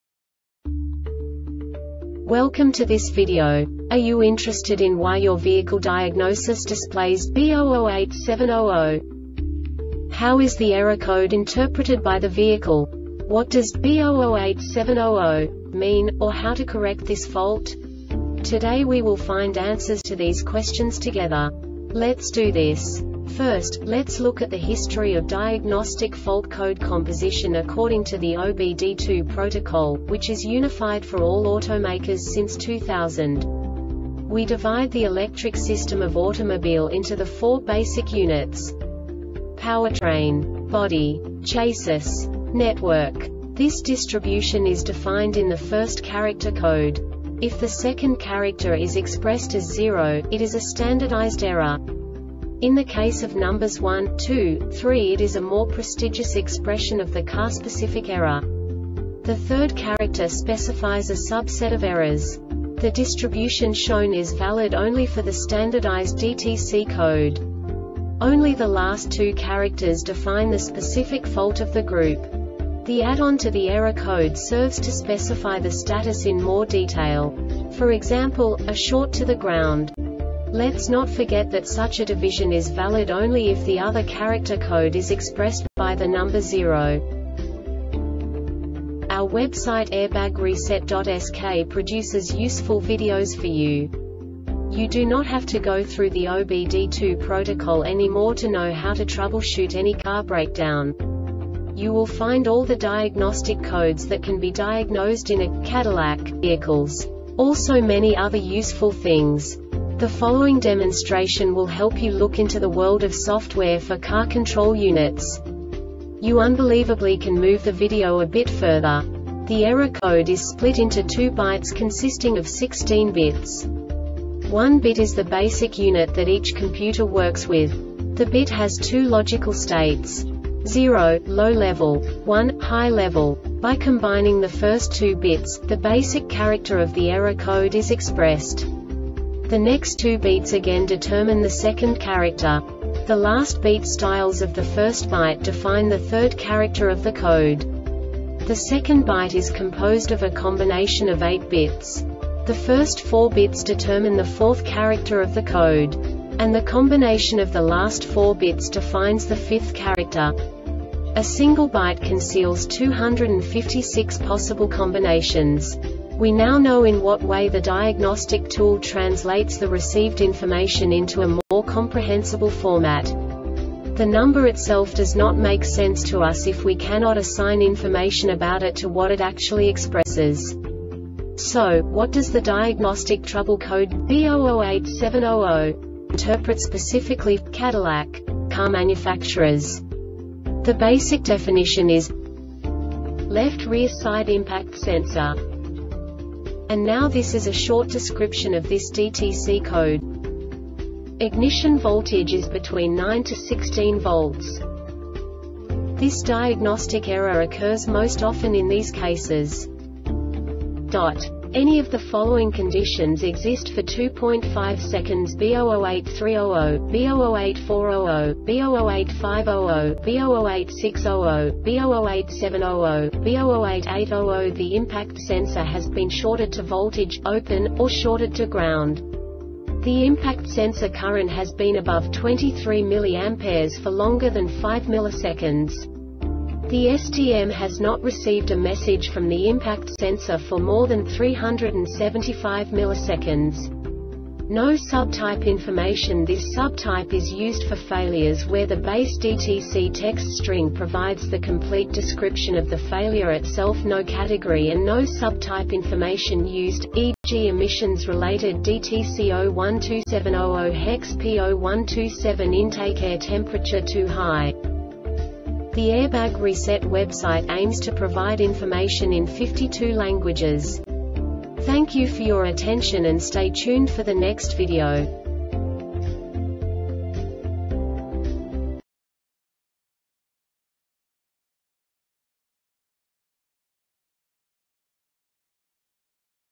DTC Cadillac B0087-00 Short Explanation
Contents:
0:21 Basic DTC analysis according to OBD2 protocol standard.
1:48 Insight into programming
2:58 A diagnostically understandable description of the Diagnostic Trouble Code
3:05 Basic error definition